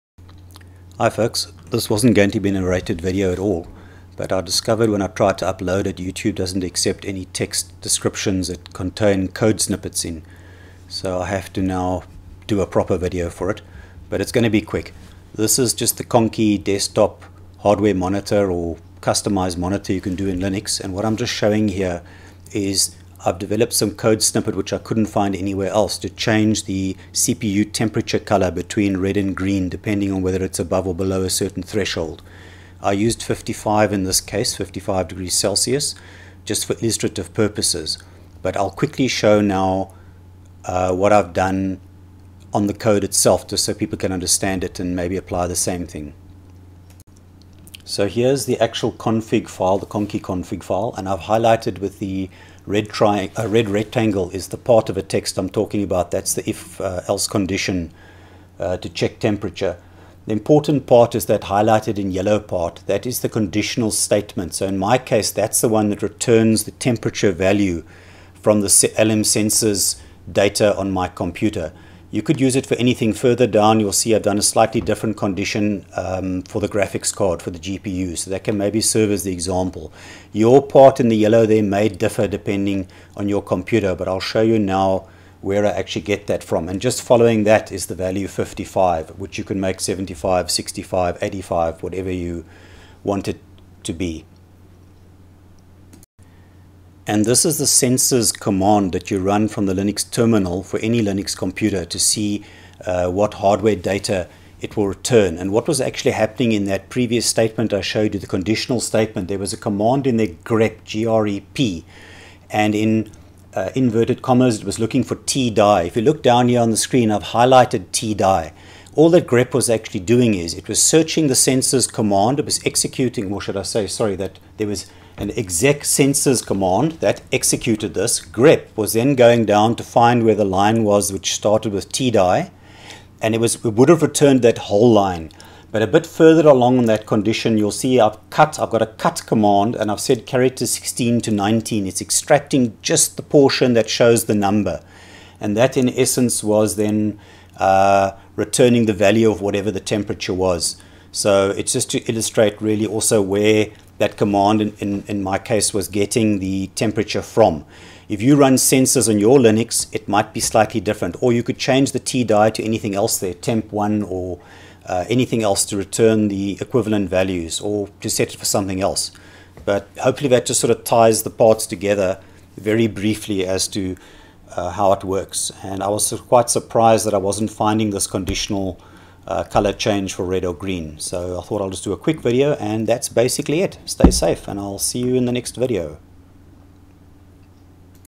Conky - How to automatically change font color for CPU or GPU temperature
This short (yes only 3 minutes) video shows the temperature colour automatically changing between red and green depending on whether the temperature value is above or below a certain threshold. In this case, I set it to 55 just for illustrative purposes, but you could make that 75 or any other value.

Intro showing effect 00:00
Code snippet 00:45
Sensors source data 01:35